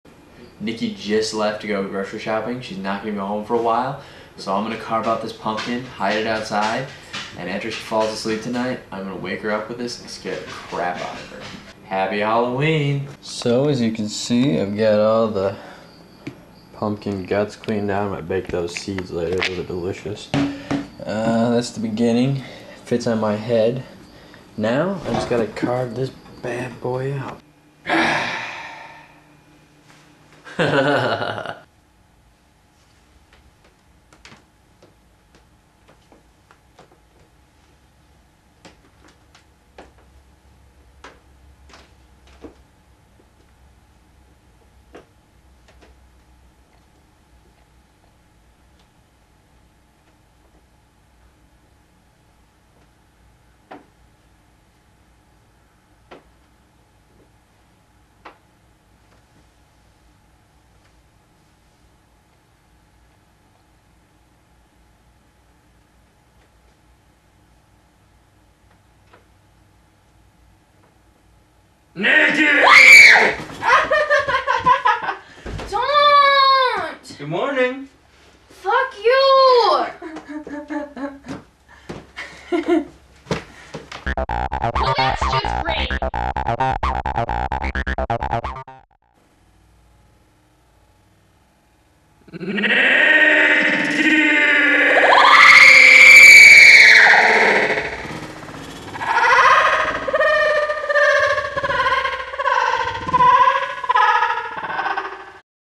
Pumpkin Halloween Scare Prank
In the spirit of Halloween John carves up a scary pumpkin and scares  Nikki while she is sleeping!

Nikki and John are a fun loving prank couple who have a few different Youtube channels. Check them all out below!